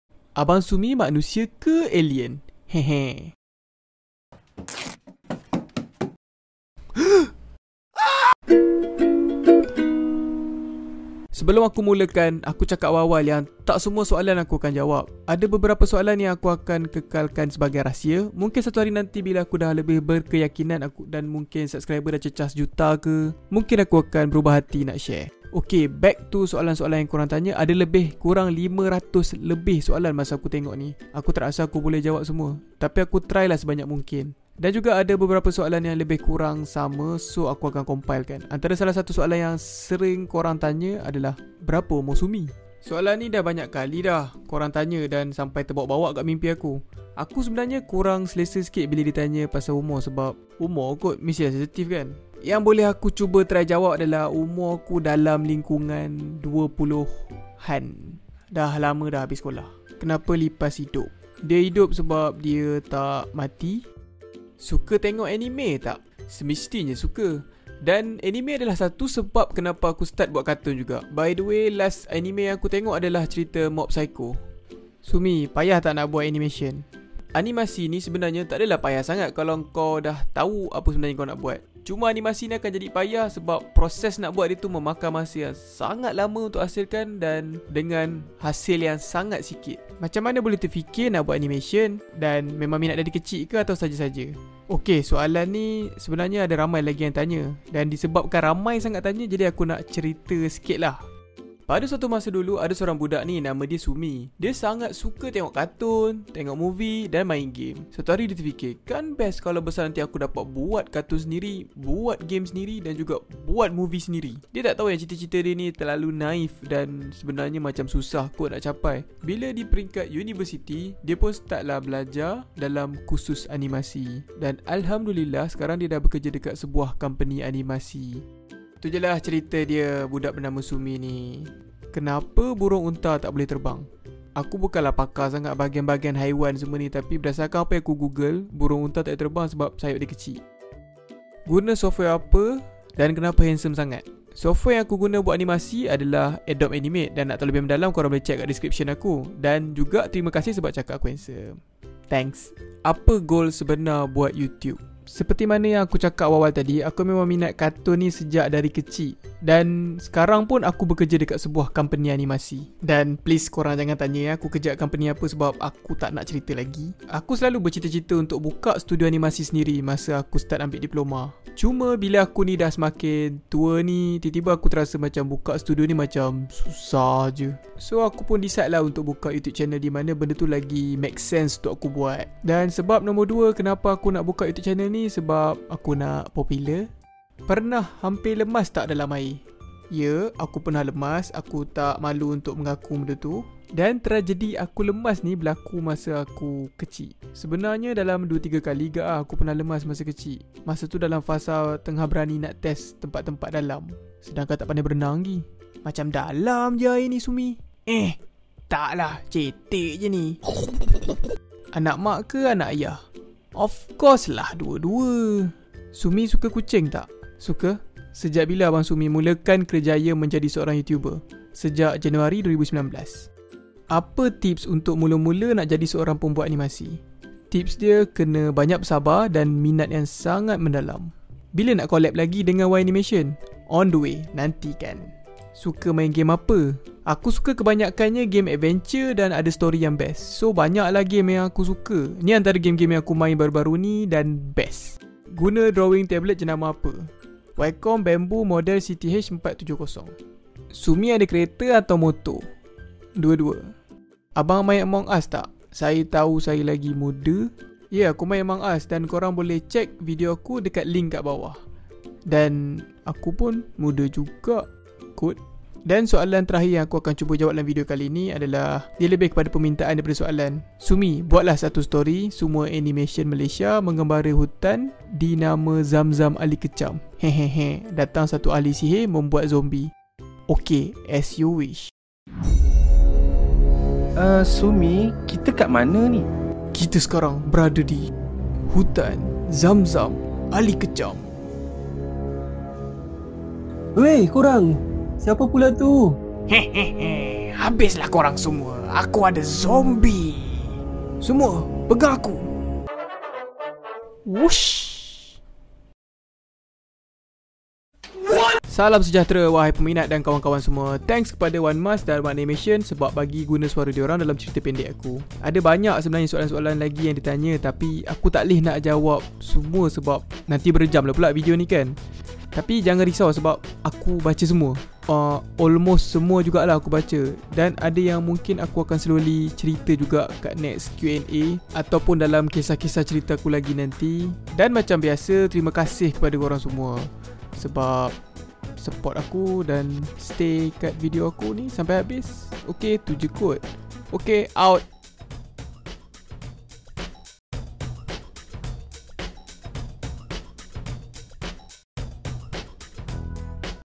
(Q&A) Soalan Pelik Korang Tanya Aku.
Soalan - soalan yang korang tanya aku, dan aku cuba jawab dengan seikhlas hati.

Software Yang Digunakan :
Toonboom Harmony
Adobe Animate
Audacity
Adobe Premier Pro
Studio Clip Paint

Sumie Azil